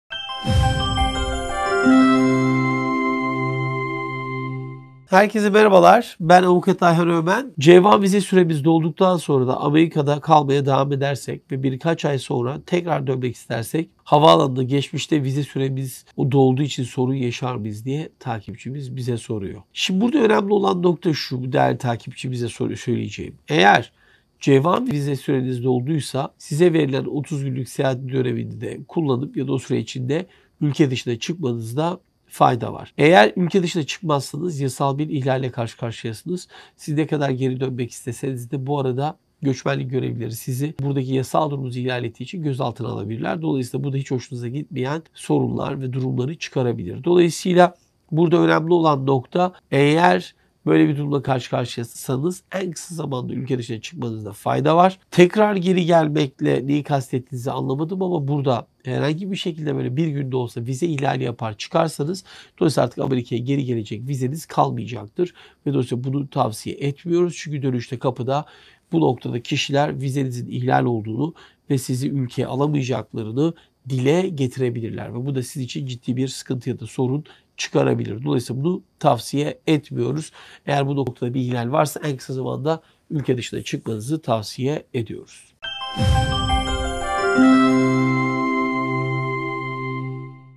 ABD VİZE SÜRE İHLALİ SONRASI AMERİKA’YA GİRİŞTE SORUN YAŞAR MIYIM ?
Soru: J1 vize süremiz dolduktan sonra Amerika’da kalmaya devam edersek ve birkaç ay sonra tekrar dönmek istersek, havaalanında geçmişte vize süremiz dolduğu için sorun yaşar mıyız ?